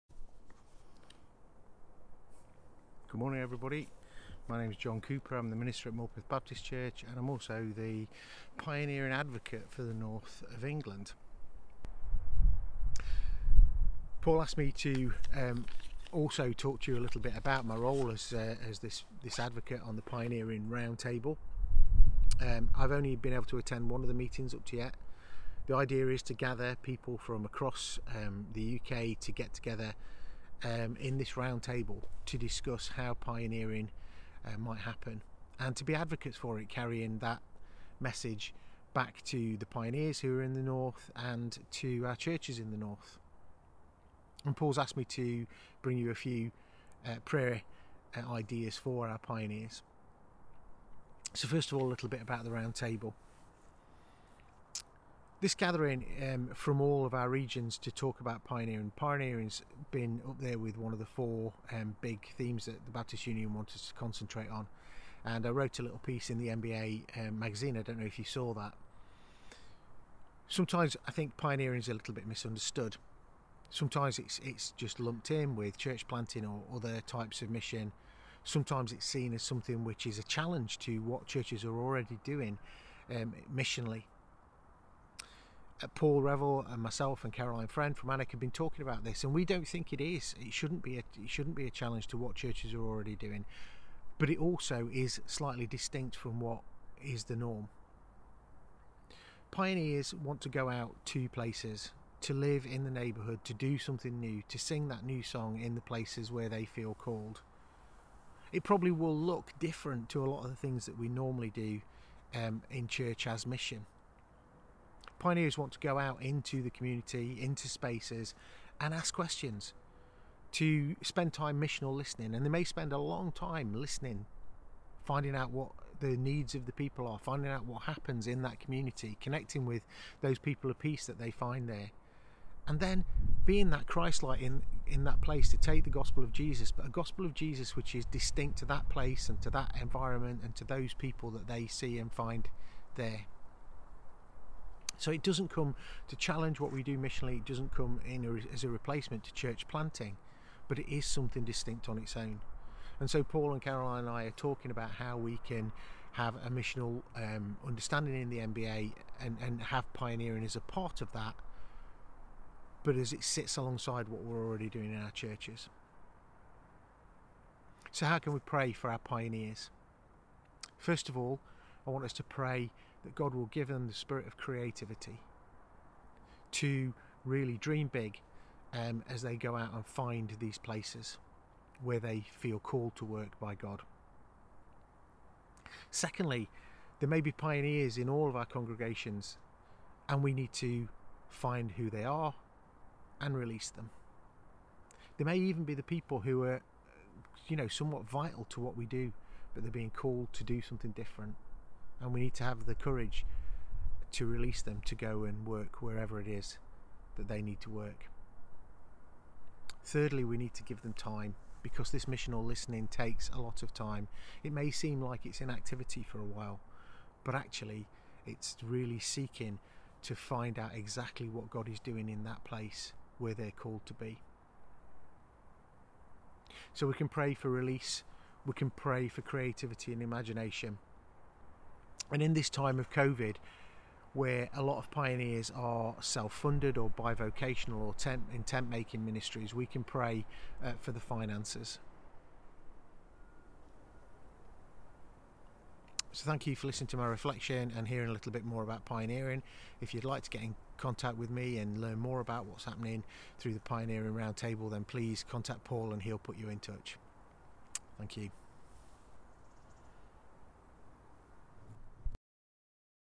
NBA Prayer Cycle: Prayer for Pioneers
John Cooper guides us in how to pray for pioneers and the work of the Baptist Pioneer Round Table